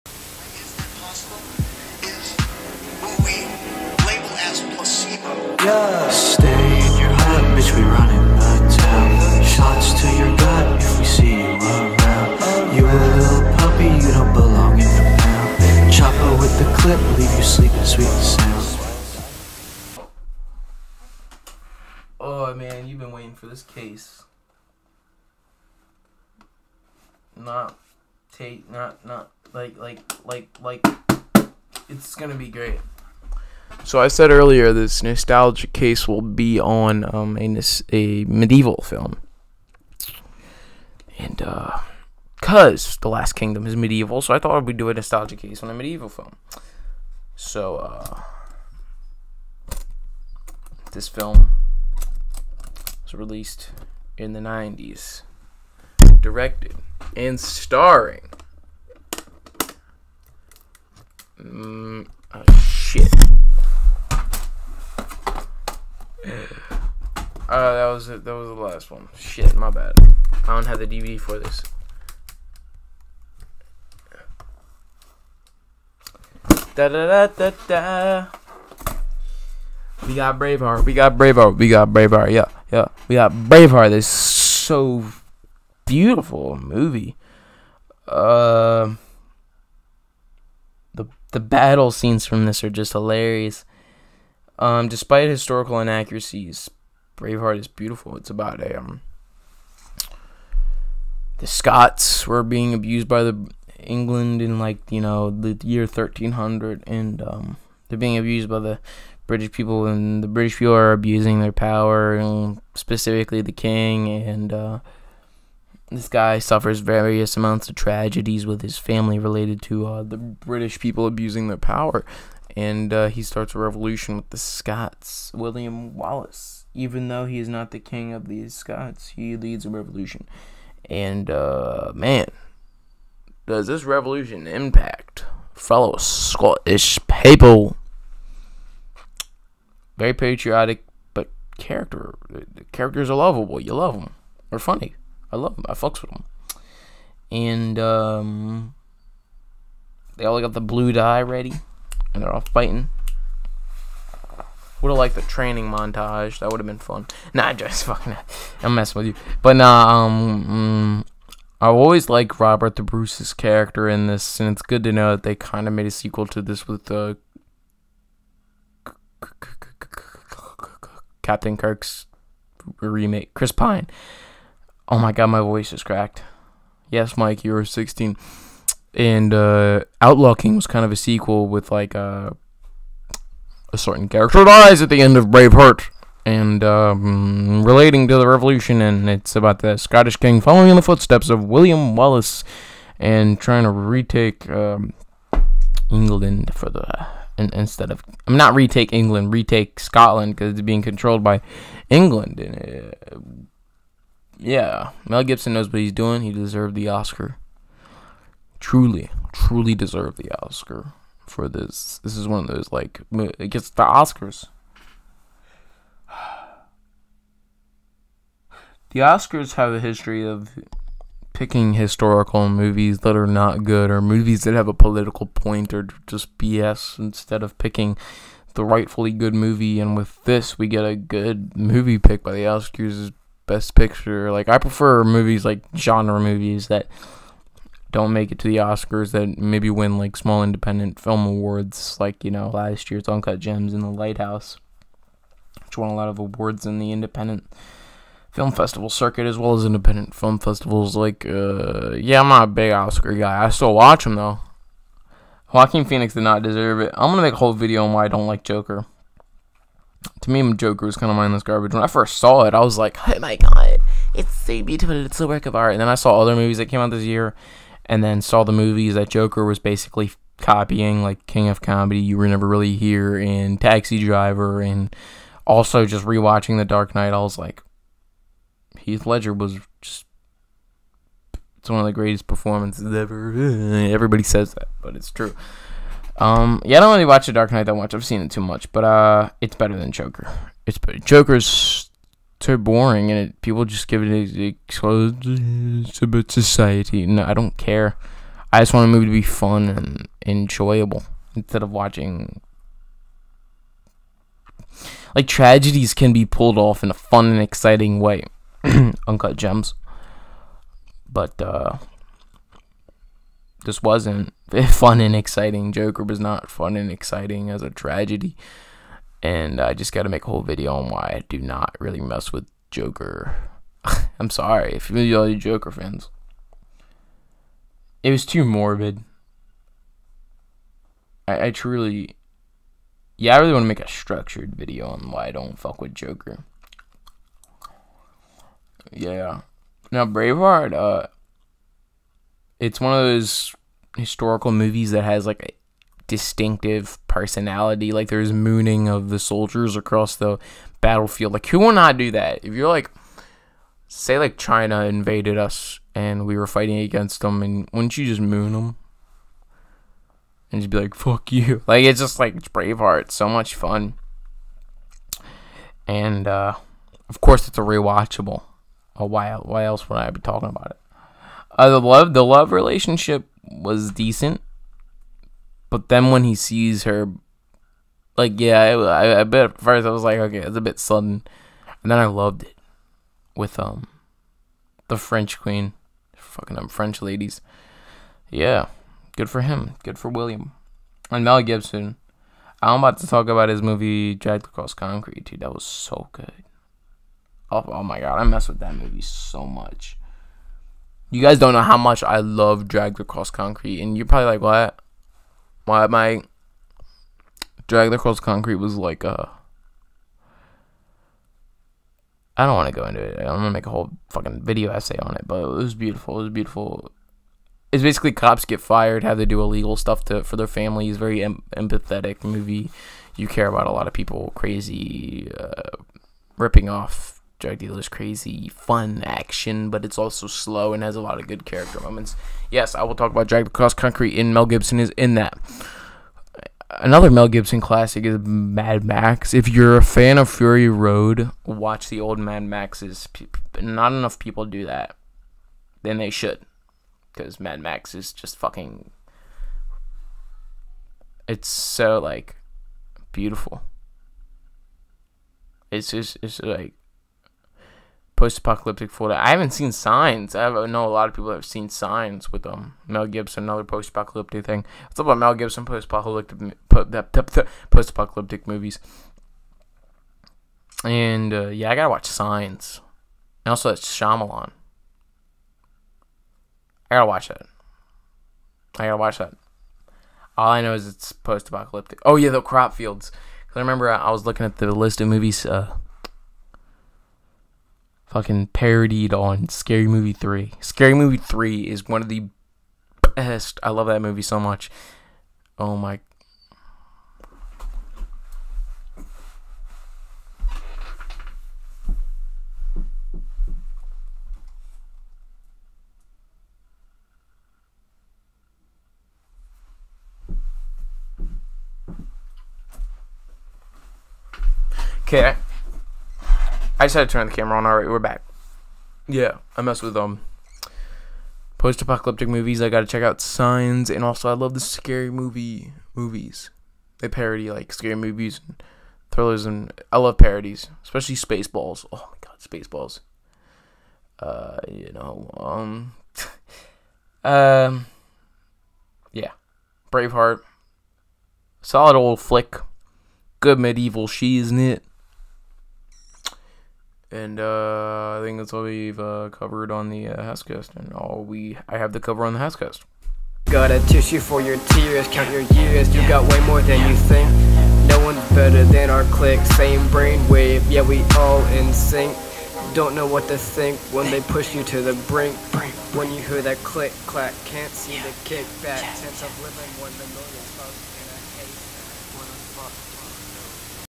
Braveheart NOSTALGIA CASE HASSCASTCLIP
This is a clip from HASSCAST 28 where i review the classic medieval  film BRaveheart Directed and starring Mel Gibson.

Letterboxd Username is mikehass_

Channel Instagram: mikehasschannel_
Personal Instagram: mikehass_
Photography Instagram: mikehasspics